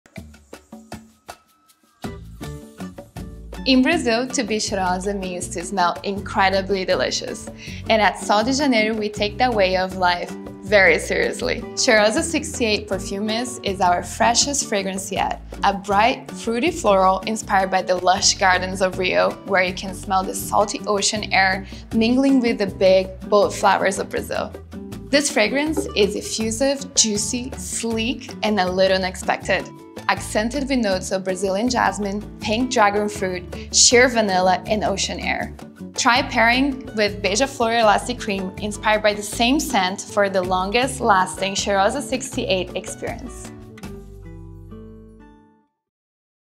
LEARN MORE: Brazilian Crush Cheirosa 68 (Beija Flor) Perfume Mist!
This fruity floral scent invites you into our lush garden by the sea—a sun-dappled oasis brimming with vibrant flora and abundant sunshine. A transportive temptation for our fragrance lovers, this immersive and uplifting scent is kissed with joy, dancing with notes of Brazilian jasmine, pink dragonfruit, ocean air, and a sheer touch of Tropicália vanilla. Every sensorial spritz inspires endless smiles and creates connections with others.

CHEIROSA 68 FRAGRANCE NOTES:
TOP: Pink Dragonfruit, Lychee Essence
MID: Brazilian Jasmine, Ocean Air, Hibiscus
DRY: Sheer Vanilla, Sun Musk

Heighten the scent experience with Beija Flor™ Elasti-Cream. This collagen-rich, elasticity-boosting body cream kisses every inch of your skin with the same fresh and bright scent. Use them together to be cheirosa even longer!